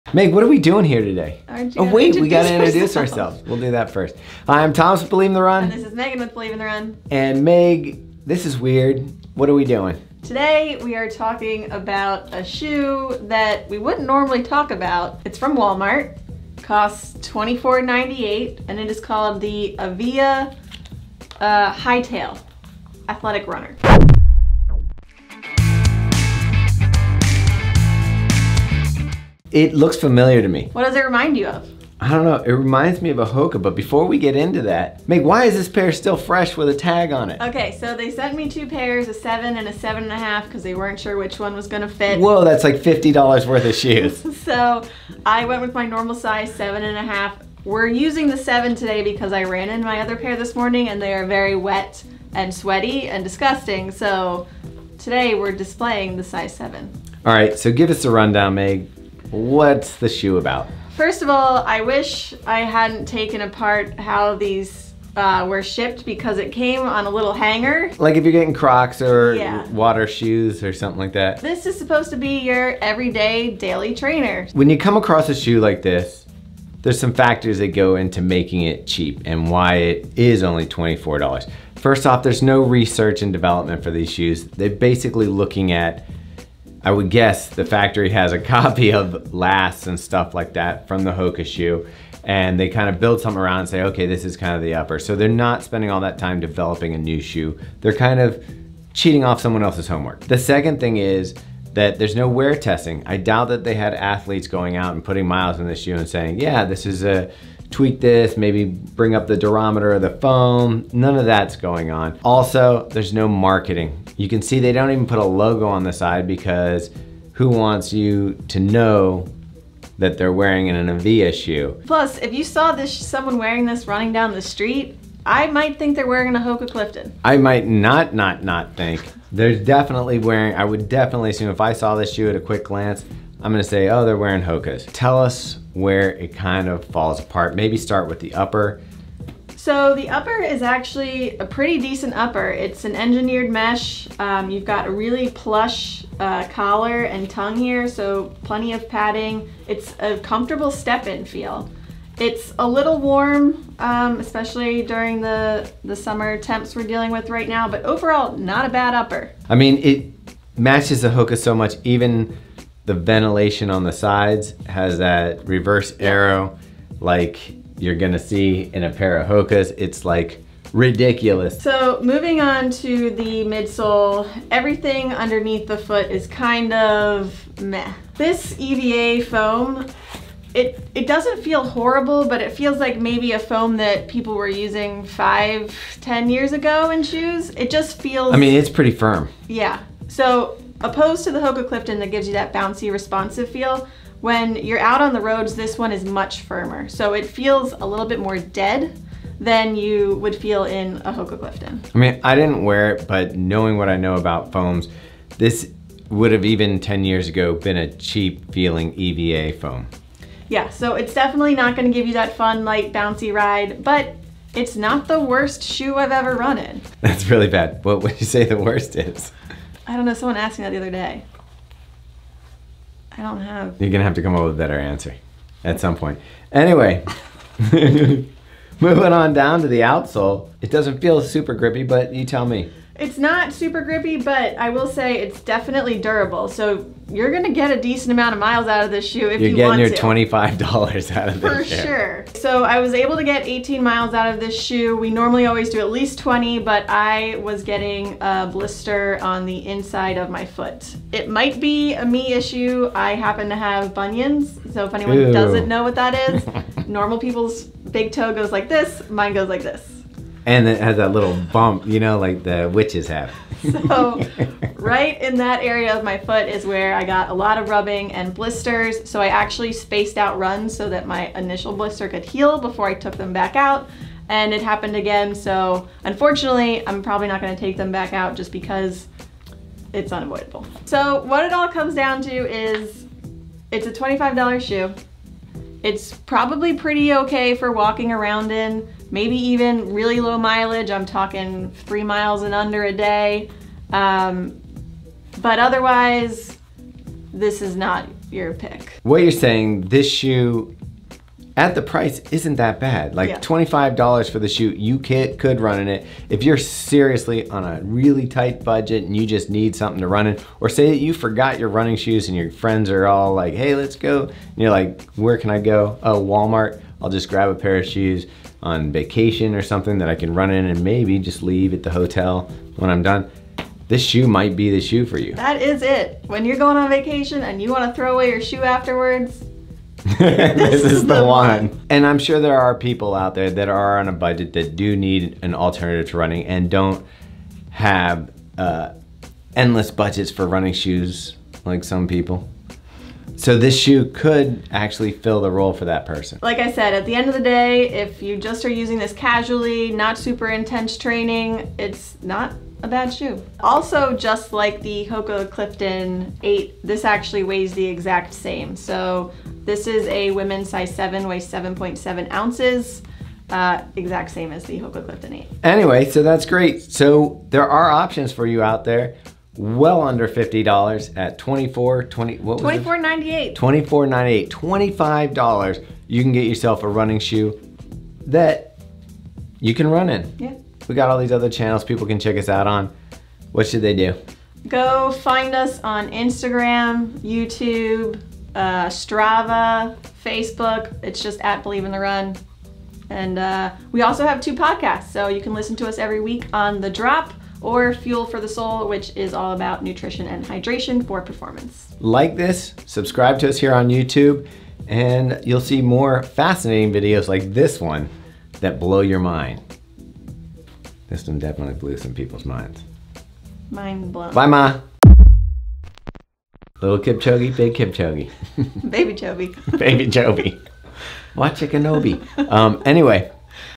AVIA Hightail Athletic Runner | Does Price Really Matter? | FULL REVIEW
Disregard the fact that the Avia Hightail Athletic Runner is a complete and obvious design rip of the HOKA Clifton 6. What we want to know is this – does price matter? Can a $25 shoe actually provide a solid ride and performance comparable to shoes five times its price? Meaghan took the shoe out for a handful of runs (including a 10-mile long run). Available now for $25.